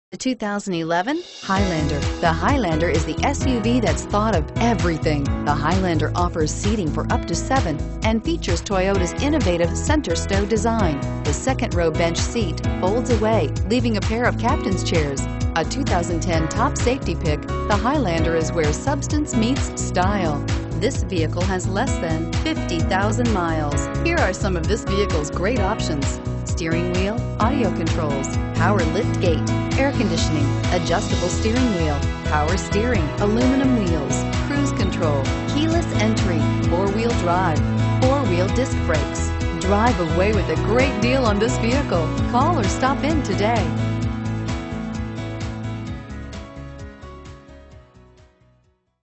2011 TOYOTA HIGHLANDER Anderson, IN 6P3259
2011 TOYOTA HIGHLANDER Anderson, IN
Stock# 6P3259

HEATED LEATHER INTERIOR!! MOONROOF/SUNROOF! ONE OWNER BOUGHT HERE NEW!! AWD! Move quickly! Are you still driving around that old thing? Come on down today and get into this superb 2011 Toyota Highlander! And with its unusually high ground clearance and sophisticated all wheel drive systems, this well-built, well-cared-for 2011 Highlander will get you there (and back) in the worst of weather conditions, without a lot of drama. Consumer Guide Recommended Midsize SUV.